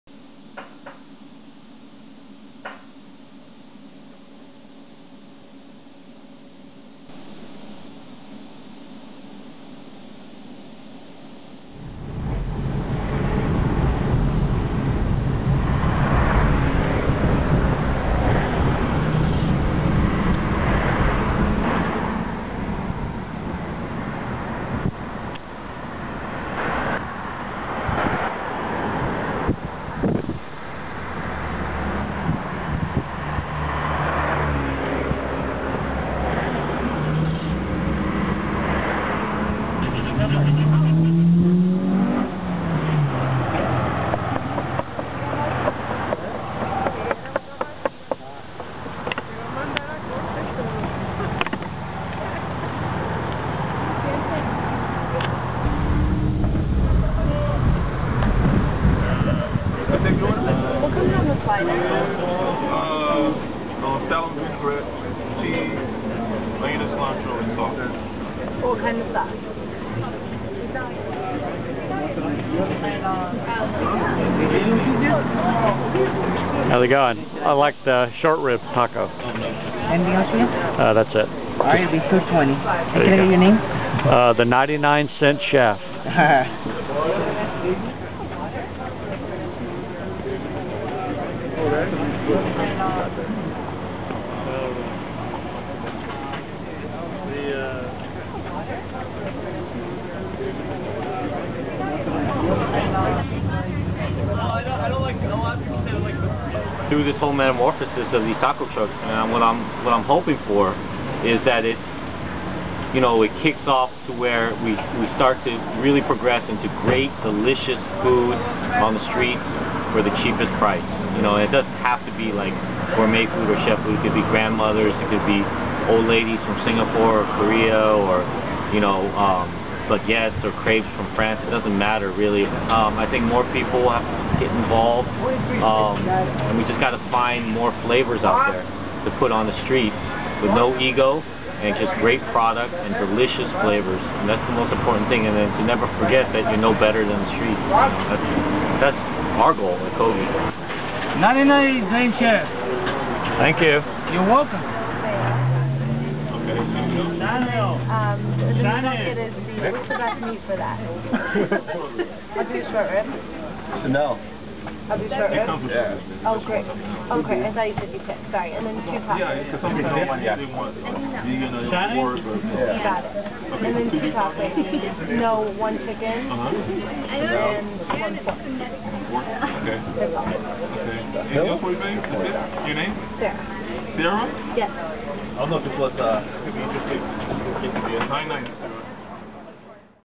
Kogi - a Korean BBQ Taco Truck
A mash-up of Mexican stylings and Korean flavors - a BBQ Shortrib Taco! The 99 Cent Chef visits L.A.'s own Kogi BBQ Taco Truck. Lotsa cool street photography & an interview. And visit The 99 Cent Chef''s cooking blog for all the tasty details